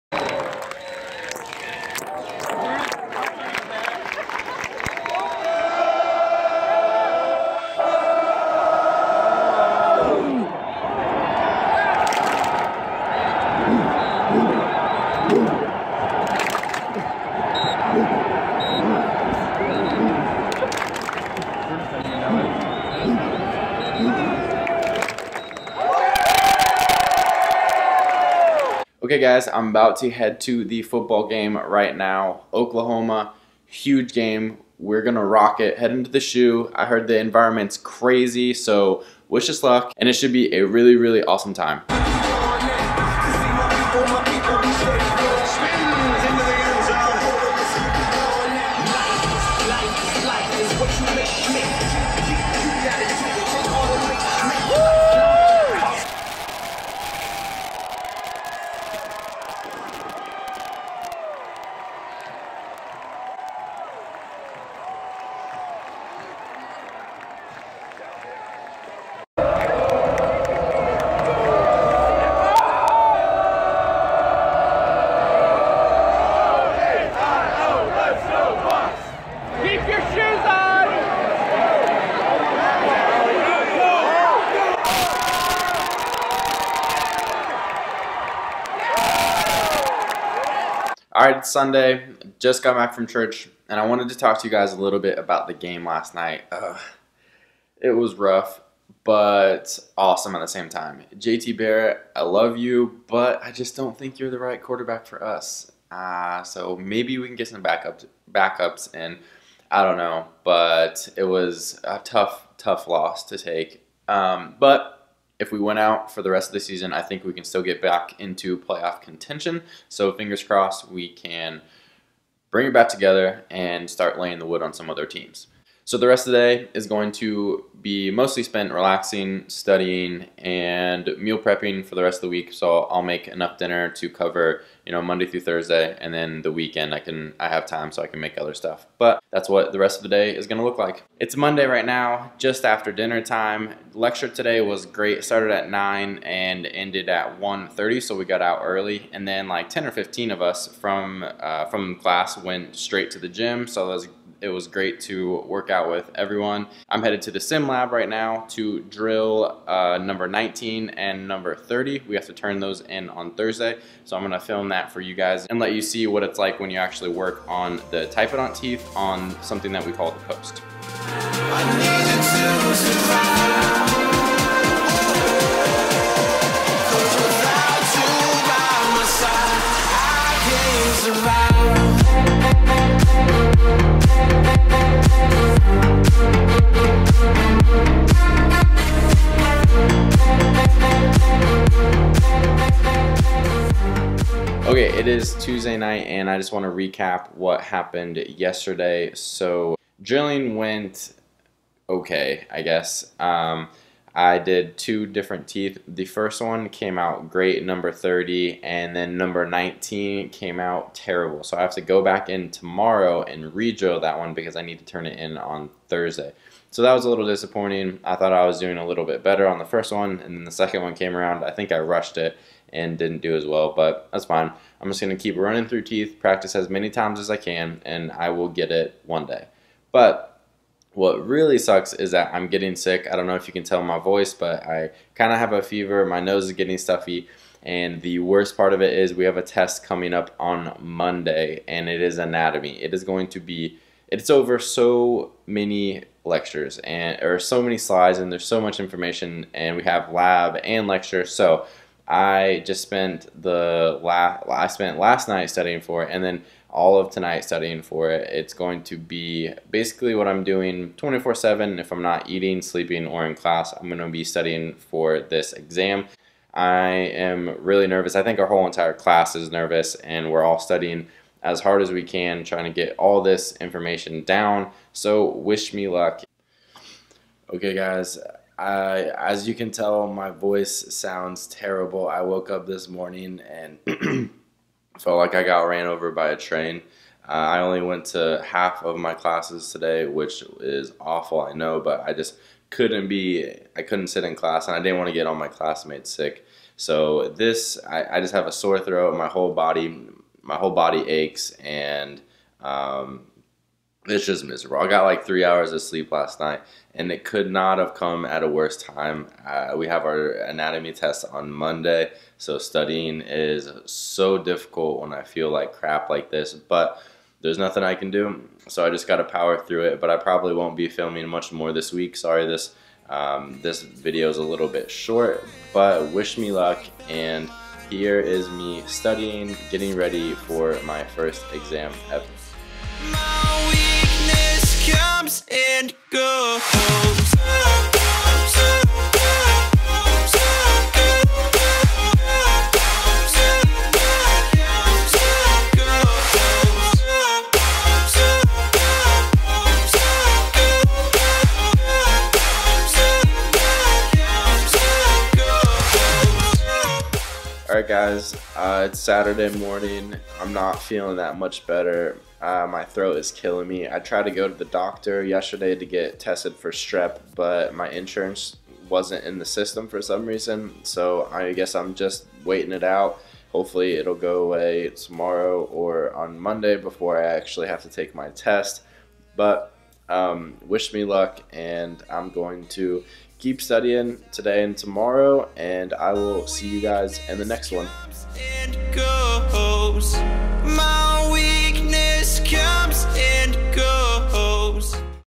SO SICK FOR MY FIRST EXAM
Trying to studying for a huge exam and I get strep!! Fingers crossed I can still do well! Dental school is rough.

Equipment:
Camera T5i